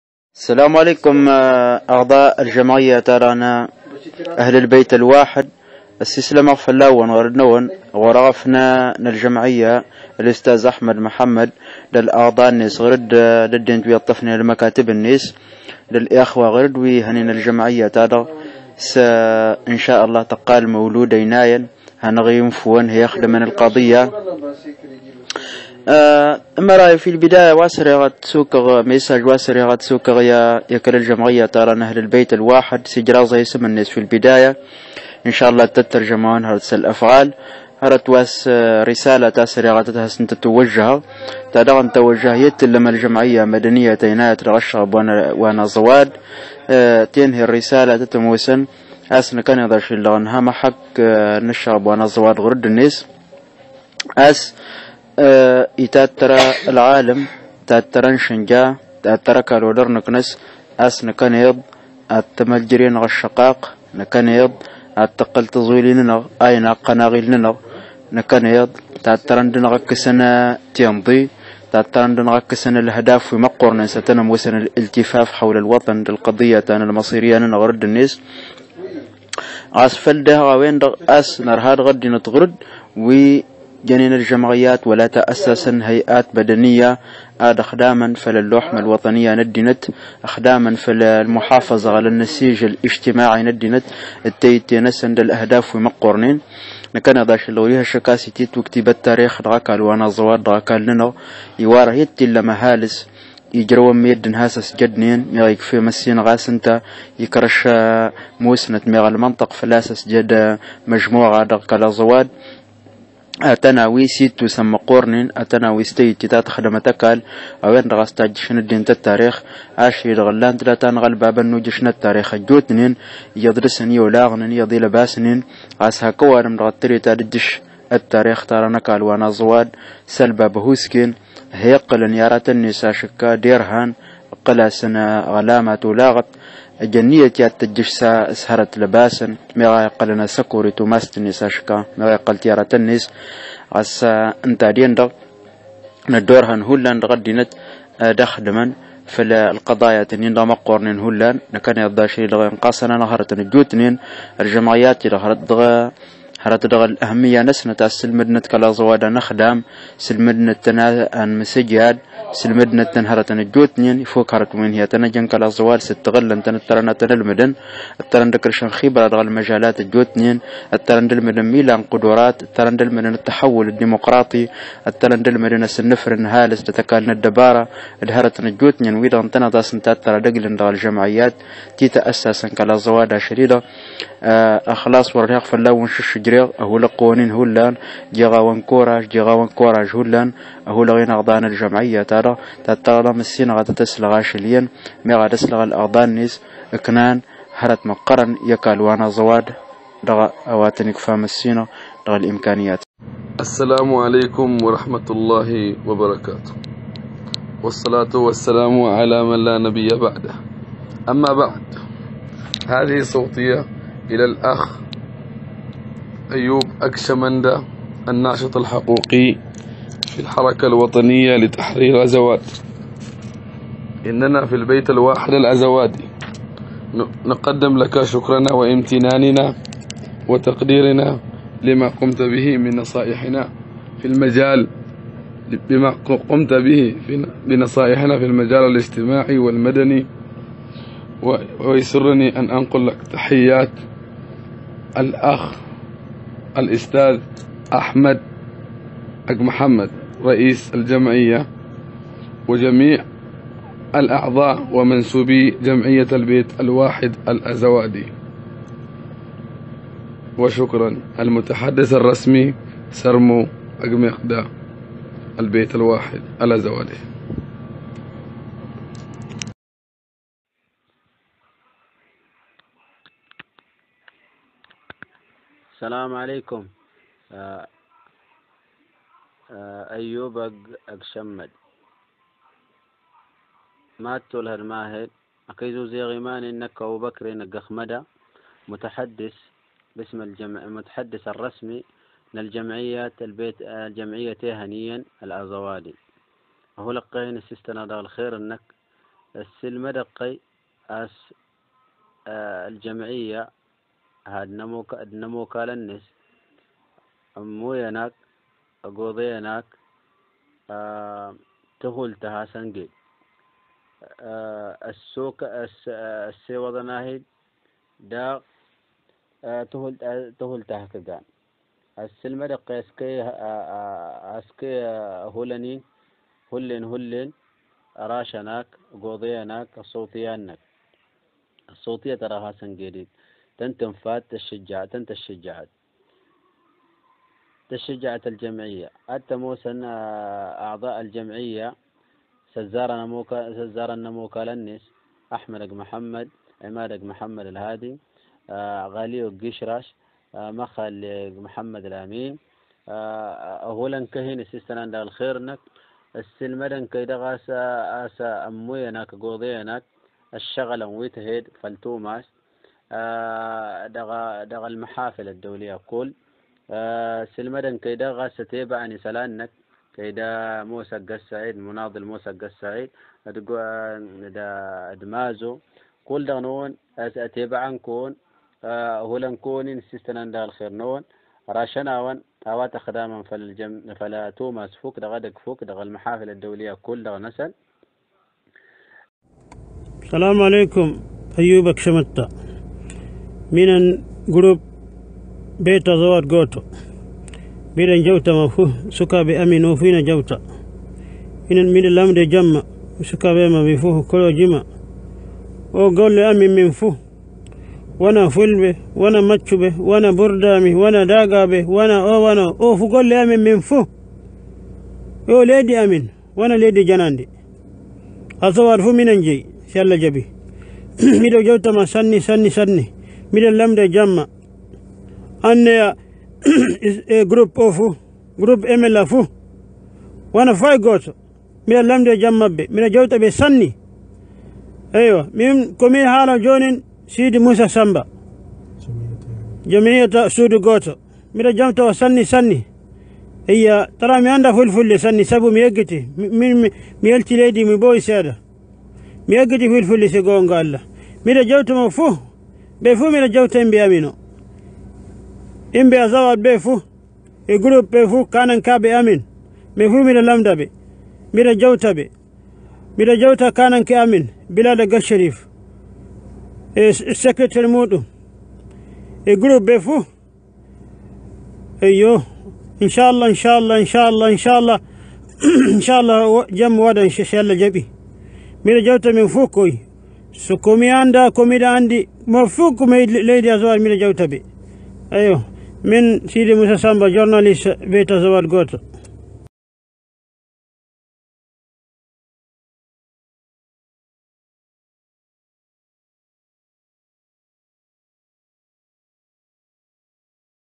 جمعة البيت الواحد الأزوادي ومداخلة عضو الحركة الوطنية لتحرير أزواد
We remind you that we are a people demanding separation, not integration, and that is why the unity of the Azawadi people and the unity of their land and the unification of its component is one of the best ways to achieve the goal and peace in Azawad